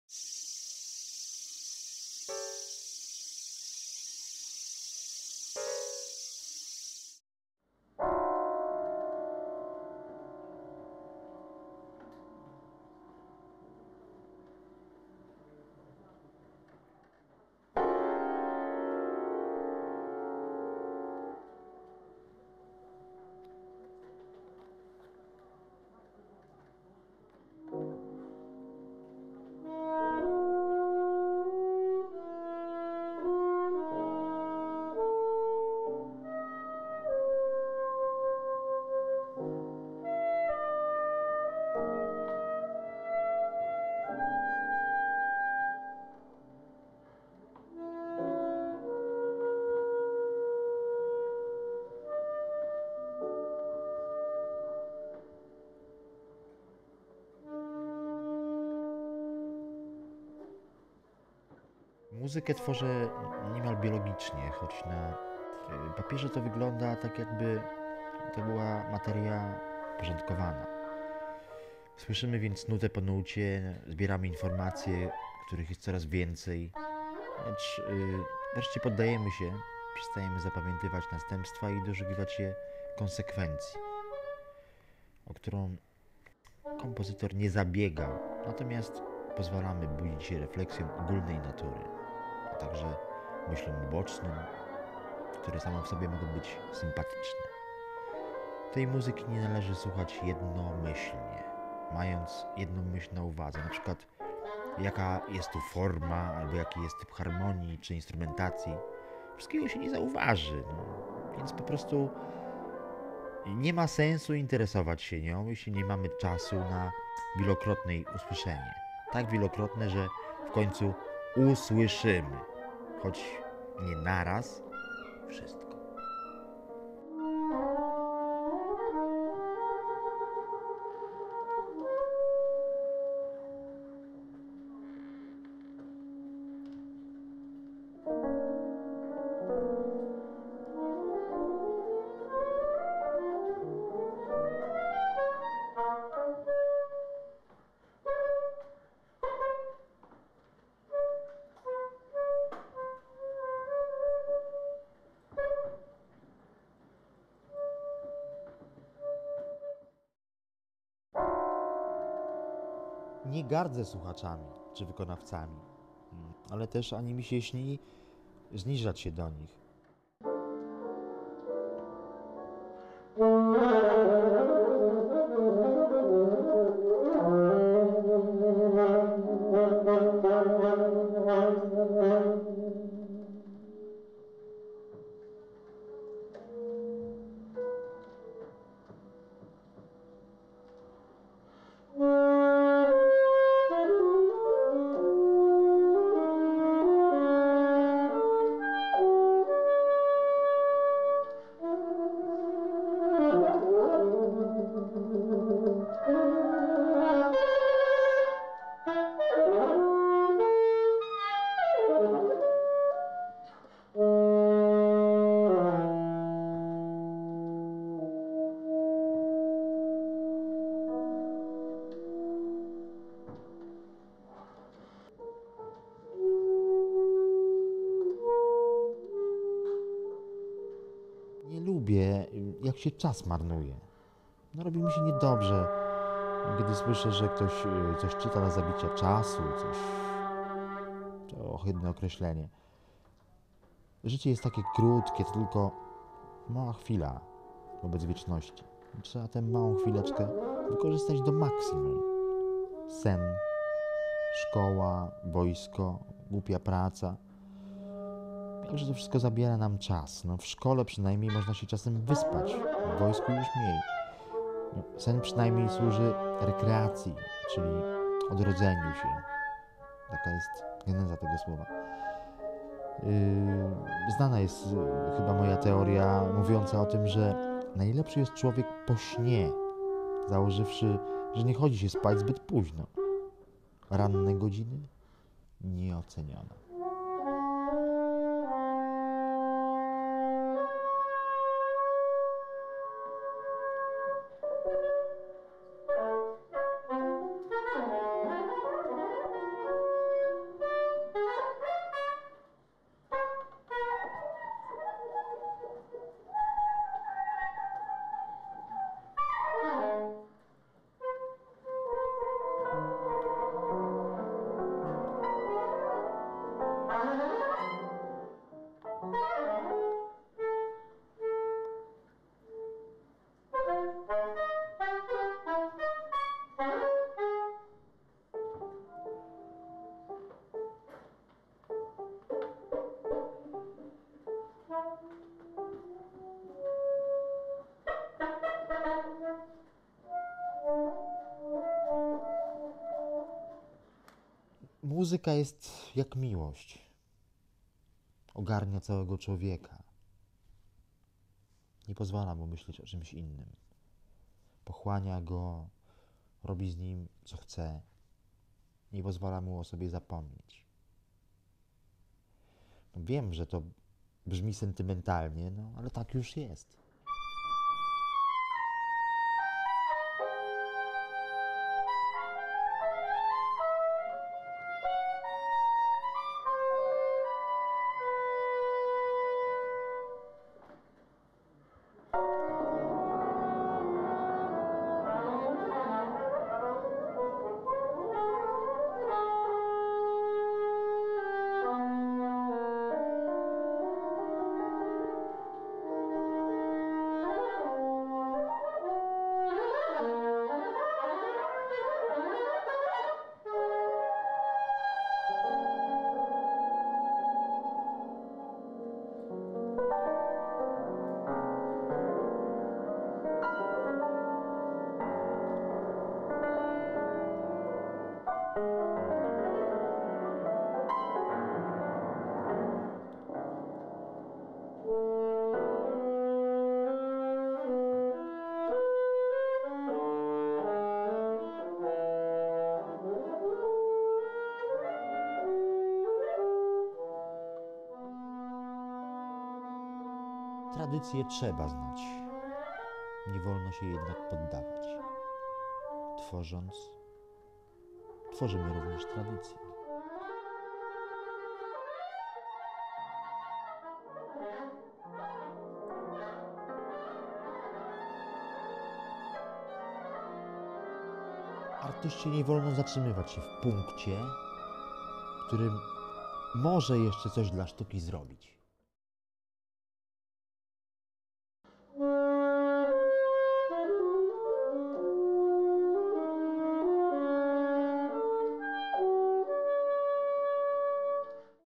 BOGUSŁAW SCHAEFFER -fragment programu "Kawałki Mózgu"
Koncert zarejestrowano podczas wieczoru poświęconemu B. Schaefferowi w sali koncertowej klubu "Mózg" jakiś czas temu.